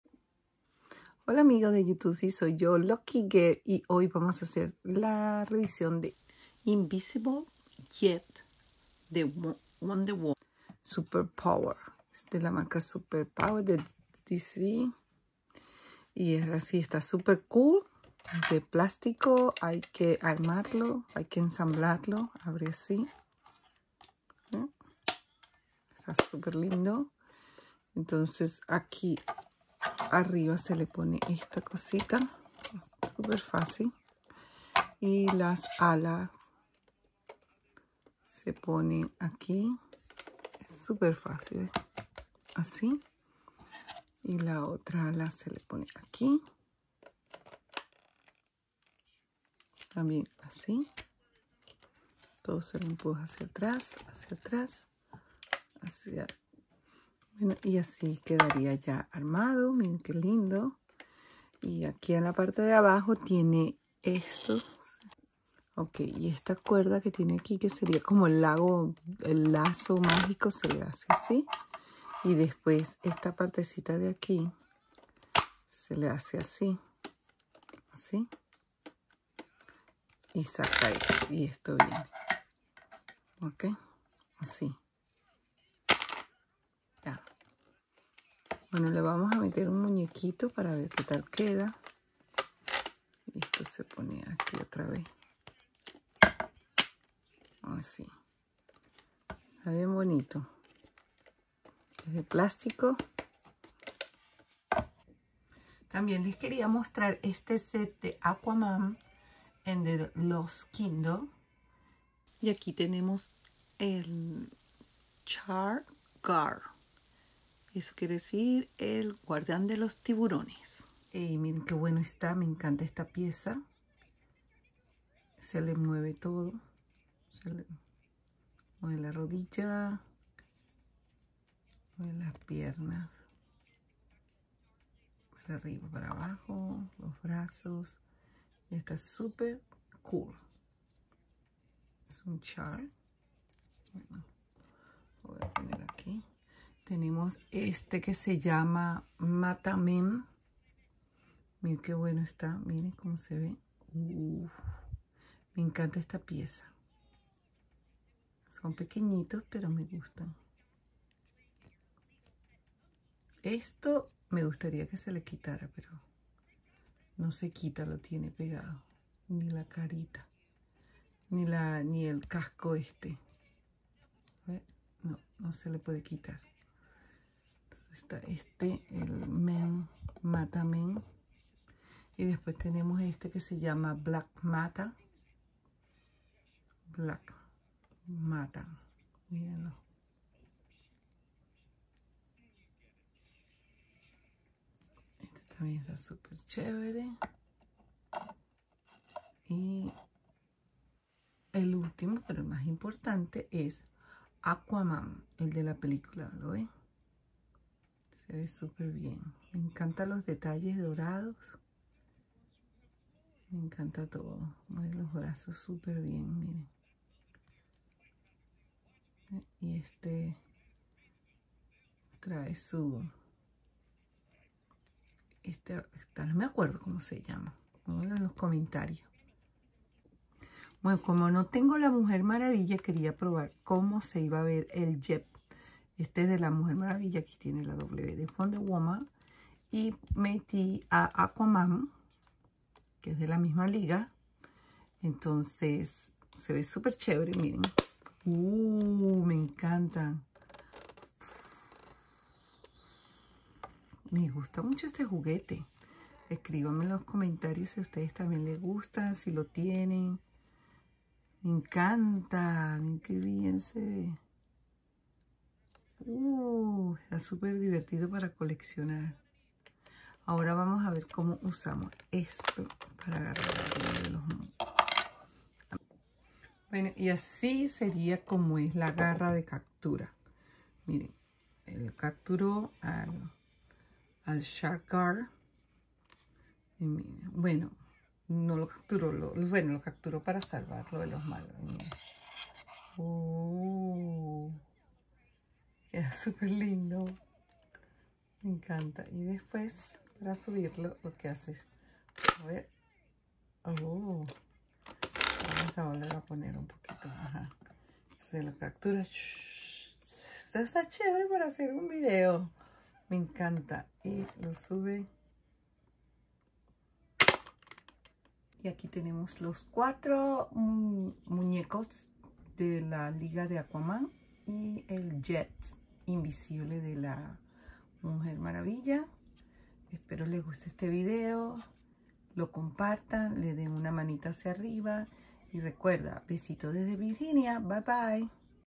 THE INVISIBLE jet Wonder Woman & AQUAMAN (colección para adultos)
Dos muy buenos piezas para agregar a su colección . El jet de la mujer maravilla y aquaman  . el jet lo compre en Walmart a unos 14$ en oferta y el aquaman en Target a 10$ en ofertas. Muchas gracias y recuerda seguirme por mis redes .